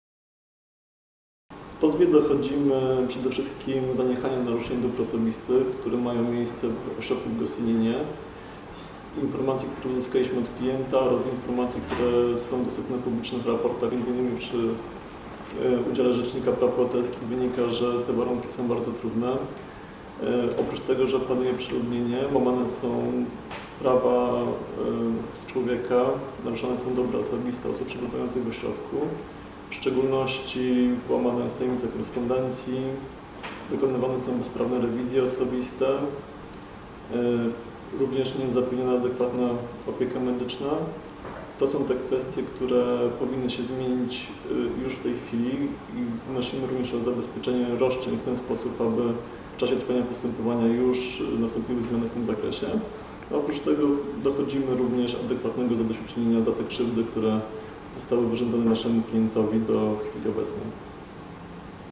Precedensowy pozew w sprawie warunków pobytu w ośrodku w Gostyninie - o pozwie
Klient Helsińskiej Fundacji Praw Człowieka wniósł pozew przeciwko Skarbowi Państwa, domagając się zadośćuczynienia za naruszenia dóbr osobistych oraz zaprzestanie dalszych naruszeń w związku z warunkami pobytu w KOZZD w Gostyninie. Utrudnianie kontaktu z rodziną, brak odpowiedniej opieki zdrowotnej i przeludniona sala to główne zarzuty, które znalazły się w złożonym 17 czerwca pozwie. Na prośbę HFPC, klienta reprezentują pro bono r.pr. Piotr Golędzinowski i adw. Łukasz Lasek z kancelarii Wardyński i Wspólnicy.

O pozwie opowiada r.pr. Piotr Golędzinowski.